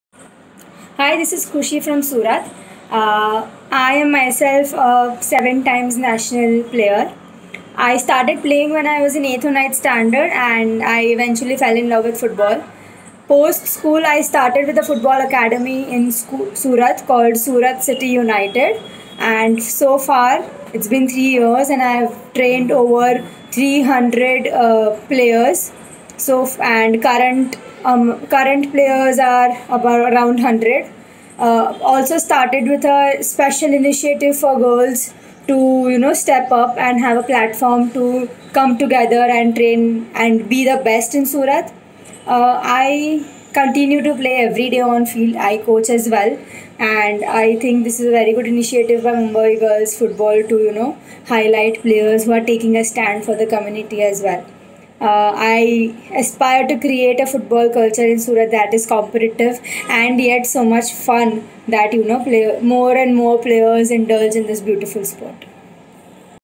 Khushi - My Game My Story
My Game My Story Series Powered By : @mumbaigirlsftbl

Contestant No 47 : Khushi Shah
Surat, Gujarat

I started playing football from 8th grade onwards. And for me to be able to pursue it for so long is only possible because of the immense support of my family and school. Without such a support system, coming this far for me wouldn't be possible.

Surat City United is a football academy based in Surat, since March 2018. I started with this academy solely because I wanted to have a platform myself to play & train with the best.

I feel blessed to have found my work in my passion at this age. Being on field is when I am most alive, and what's life without a good game?

Happy to share that we have trained over 300+ players. We have also formed a team who strive towards a common cause, which is creating a football culture.

I plan to continue to grow in quality & quantity in SuratCityUnited & myself to keep playing the game I fell in love with.